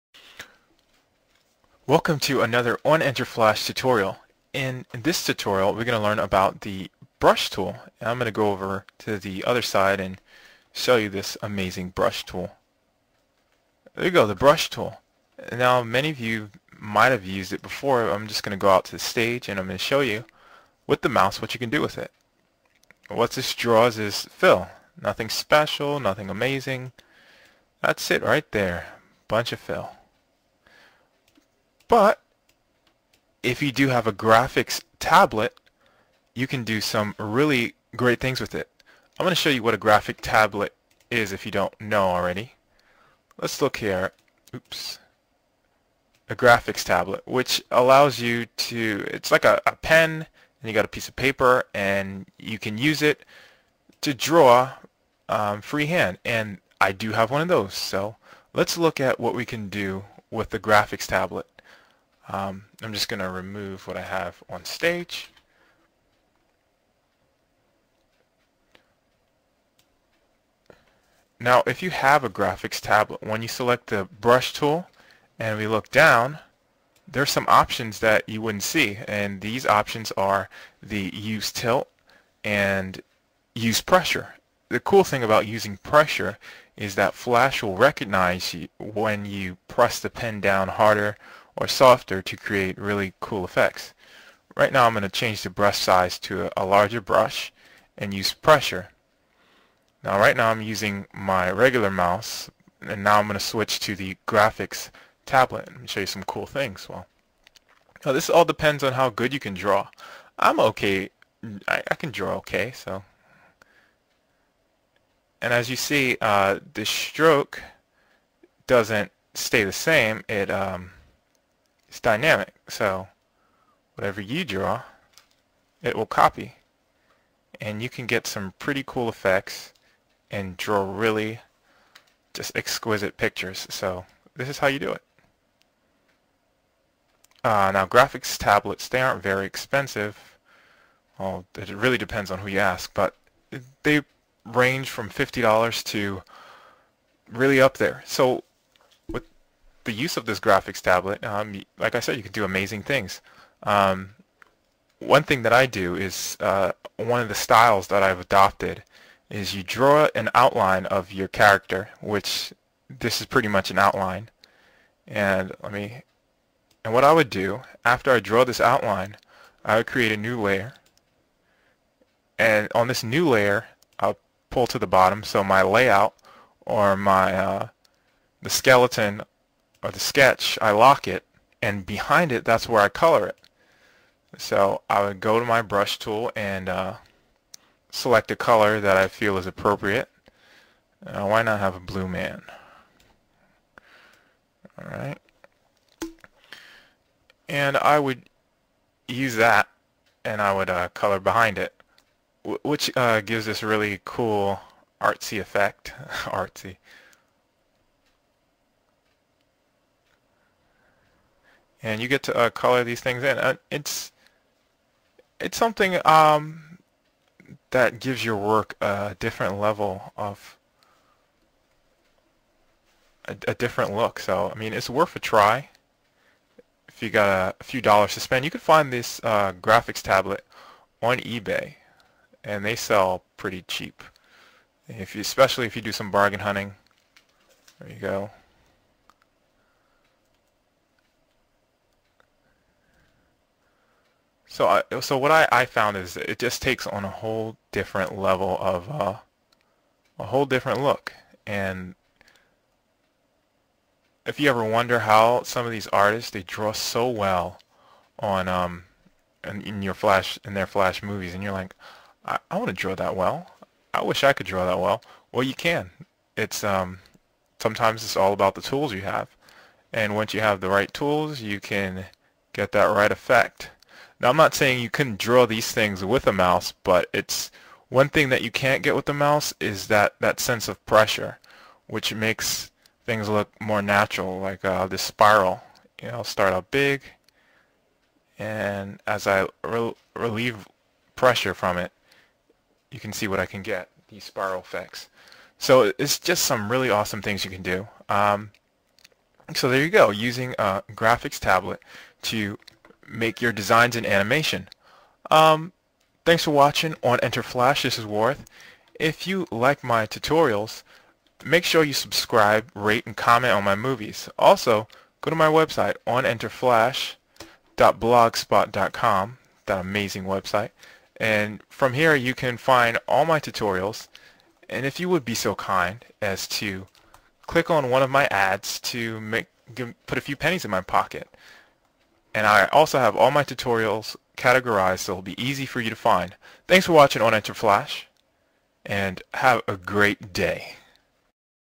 Adobe flash cs4 basics : 0104 Brush Tool
This tutorial will go into the Basics of Adobe Flash cs4 IDE. This includes the lay out tools and other parts of flash cs4. The Brush tool is for creating complex drawings in adobe flash cs4 and cs3. Also I show how to use a graphics tablet to draw in adobe flash.
-Waarith Abdul-Majeed